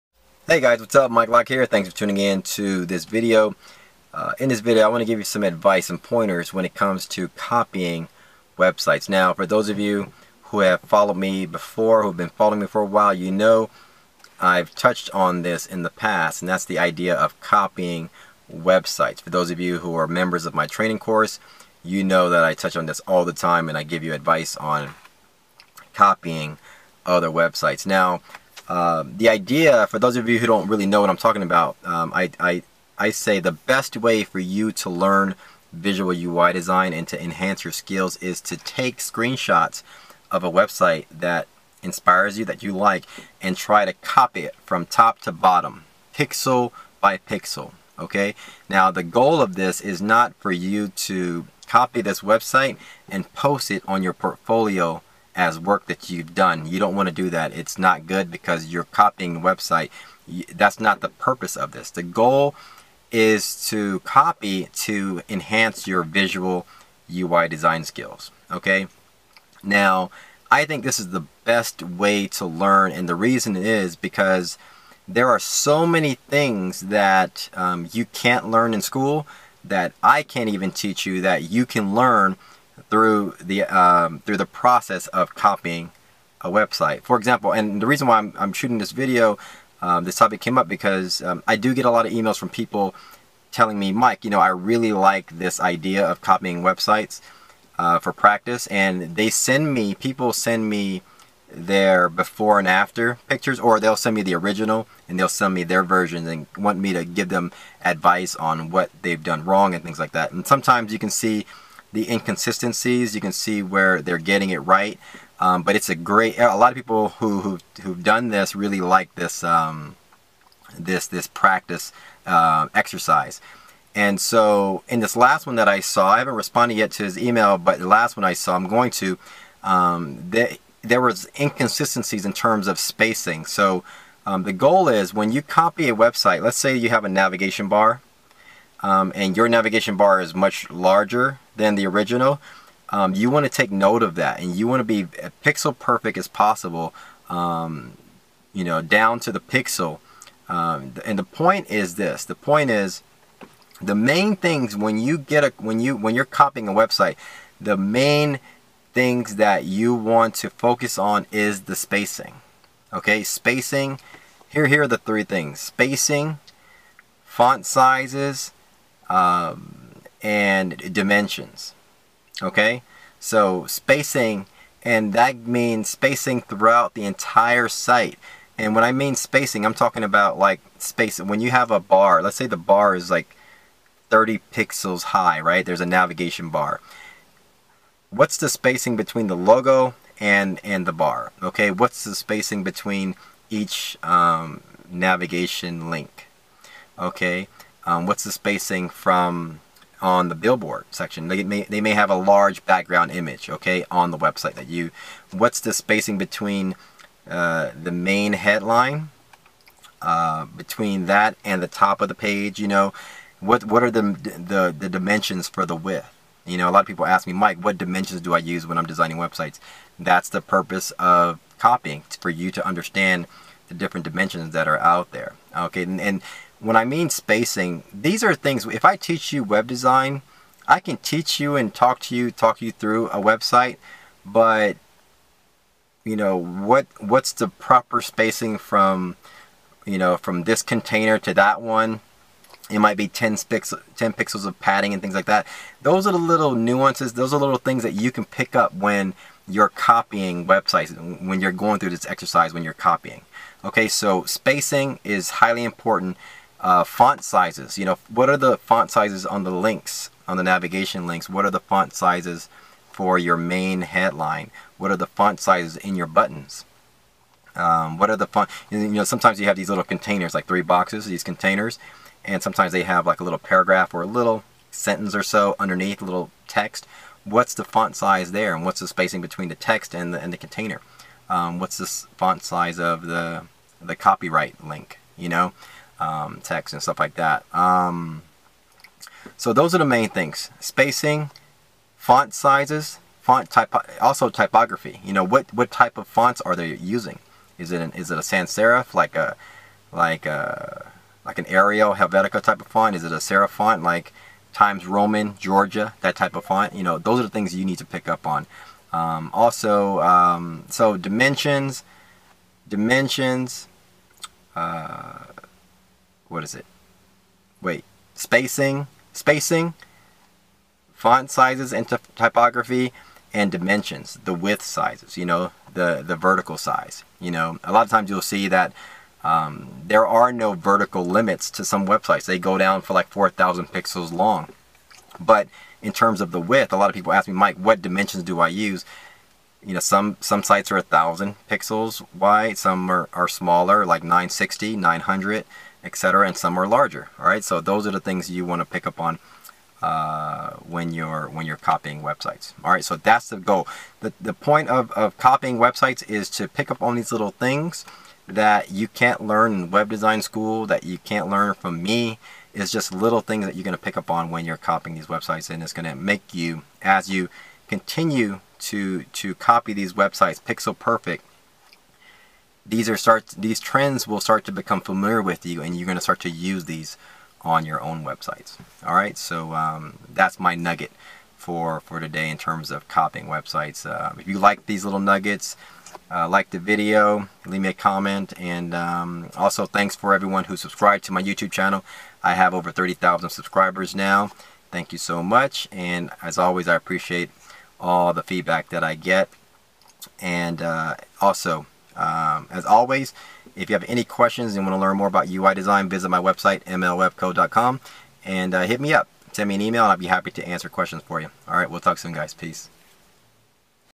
Web Design Advice on Copying Websites (for Practice)
- In this video I share some tips and advice on what to focus on when copying websites for your practice exercise. Helping you grow your Web Visual Design skills.